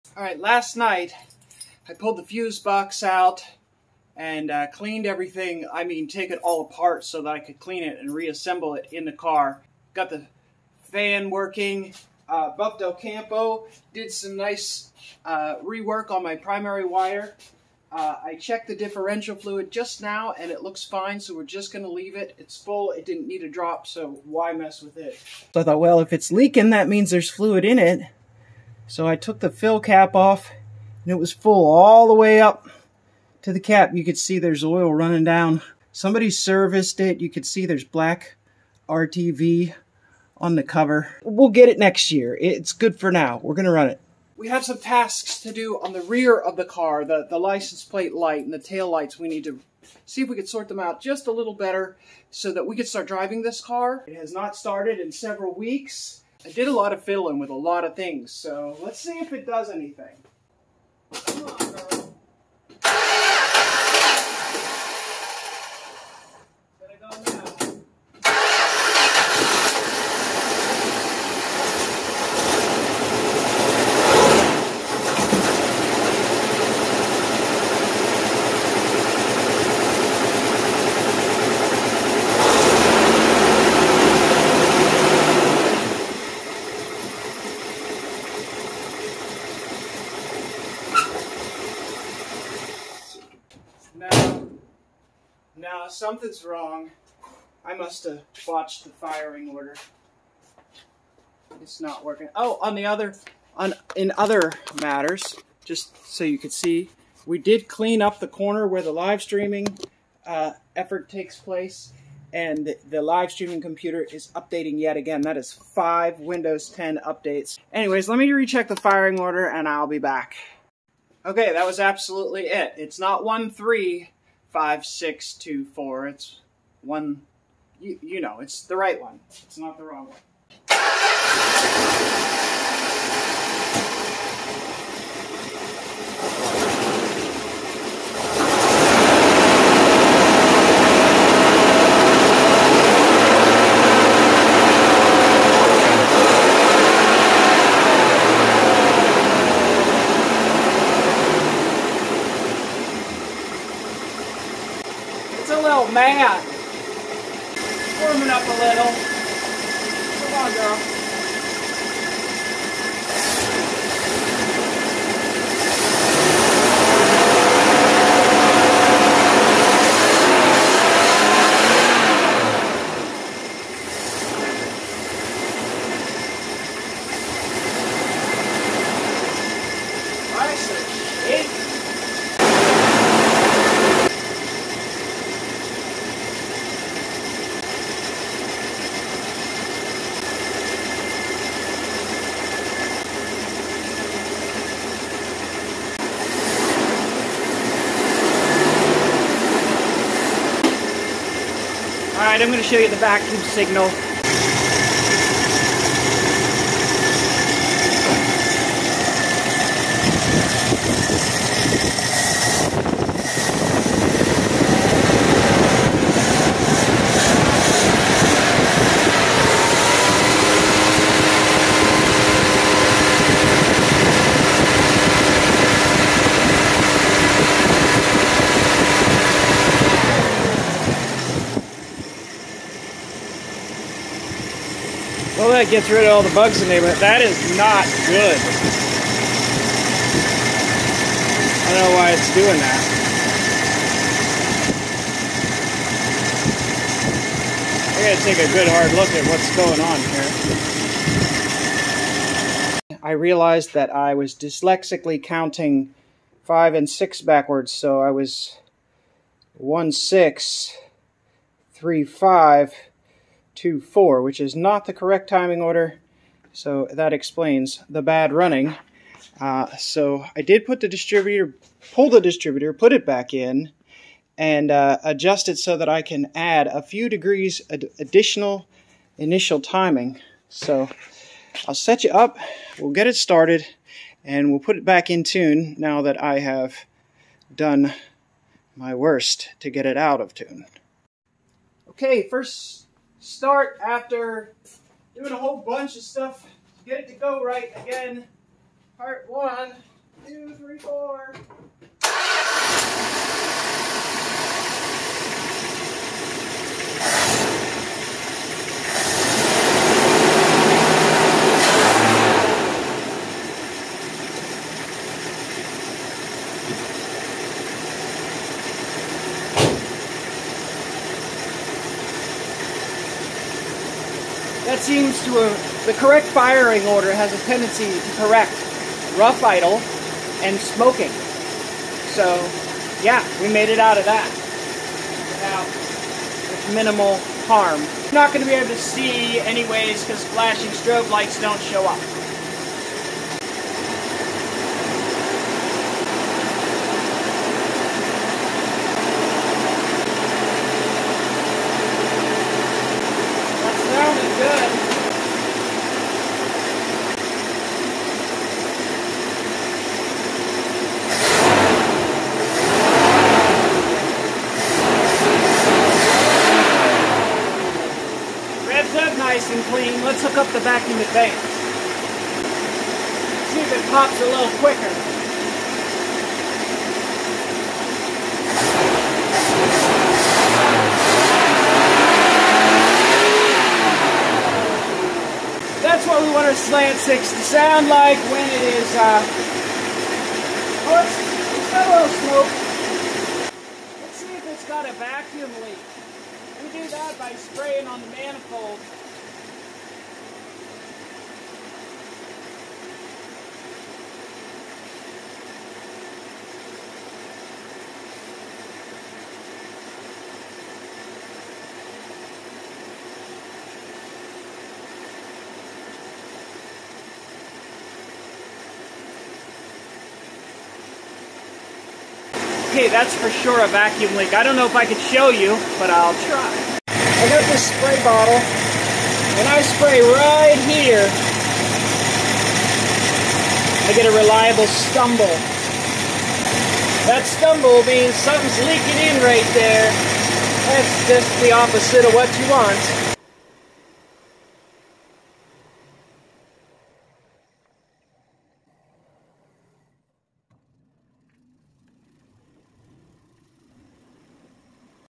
What happens when the firing order is wrong? 1966 Dodge Dart slant six.
The correct firing order for this chrysler inline six engine is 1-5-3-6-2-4. On the first start, I think I had it at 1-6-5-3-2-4. Next I corrected to only slightly better of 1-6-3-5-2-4. The engine shakes and smokes and is impossible to adjust with carb settings when the timing order is incorrect. Too many late nights and scattered attention. Now we are getting sorted. The fine tuning with timing and idle mixture will be easy now that the engine is firing on all six. You can notice by the end of the video that the engine starts and runs smoothly with much less smoke from the breather. This is just a worn out old engine, but it is getting better and is fun to work on and drive. Old cars like this are great for beginners to learn and understand the interactions of the fuel and electrical systems on internal combustion engines. Classic cars are useful and educational. Mopar or no car.